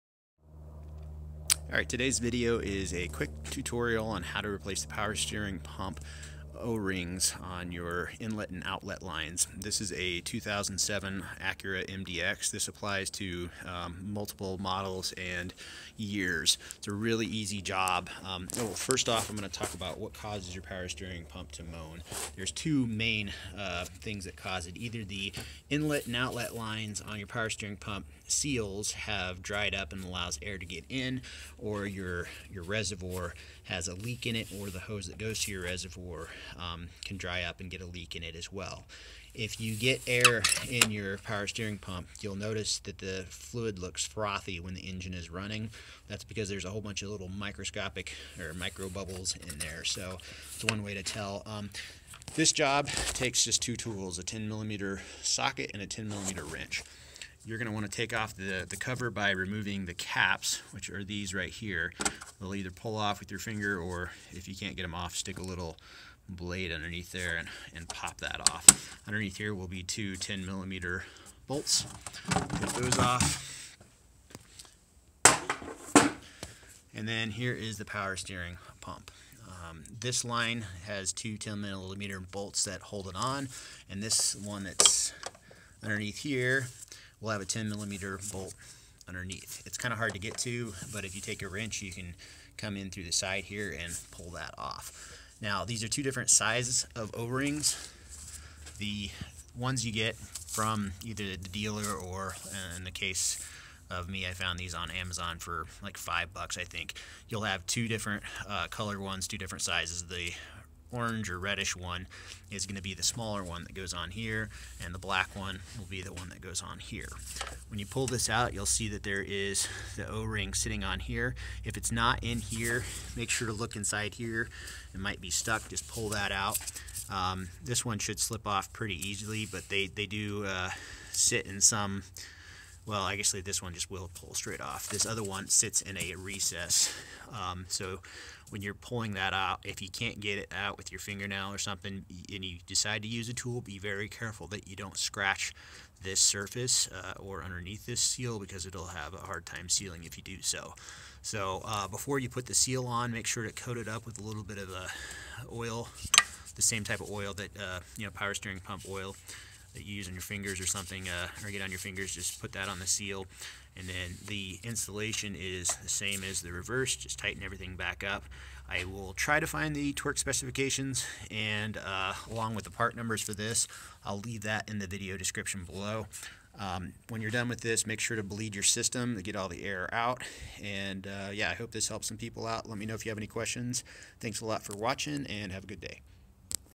Honda & Acura Power Steering Moan fix. O-Ring Replacement.
This applies to many Hondas and Acuras. This is how to fix the Power Steering Pump Moaning by replacing inlet and outlet O-rings on pump. Super simple job. This was on a 07 Acura MDX. Part numbers for this vehicle (01-08) and many others is . 91345-RDA-A01  &    91370-SV4-000
The top 2 bolts are torqued 8 Ft Lbs so I assume the bottom one is as well. Prelude, Accord, Civic, CR-V, Element, Odyssey, Pilot, Ridgeline.